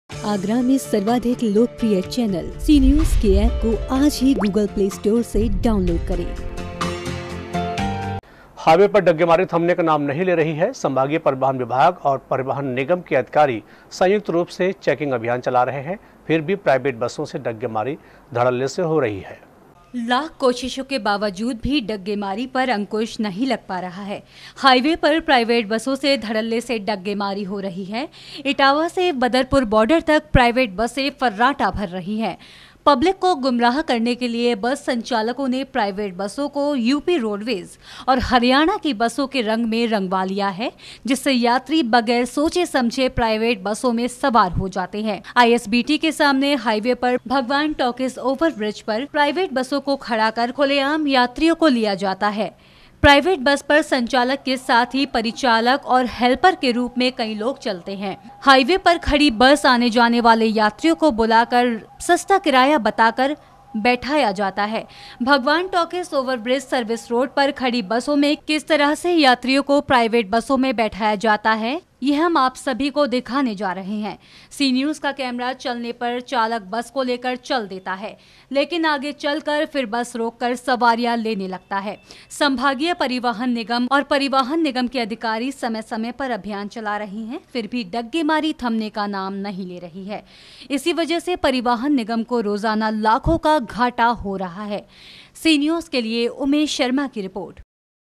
लाख कोशिश के बावजूद भी नहीं थम रही प्राइवेट बसों से डग्गामारी - Sea News Agra
हाईवे पर डग्गामारी थमने का नाम नहीं ले रही है संभागीय परिवहन विभाग और परिवहन निगम के अधिकारी संयुक्त रुप से चेकिंग अभियान चला रहे हैं फिर भी प्राइवेट बसों से डग्गामारी धड़ल्ले से हो रही है